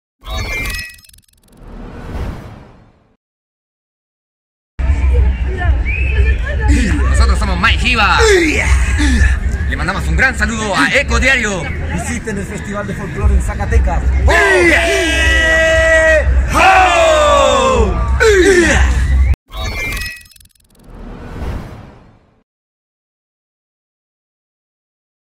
Las culturas se unen en el FZFI2019
FZFI2019 Distintas culturas unen su pasión por la música y el baile en el Festival Zacatecas del Folclor Internacional 2019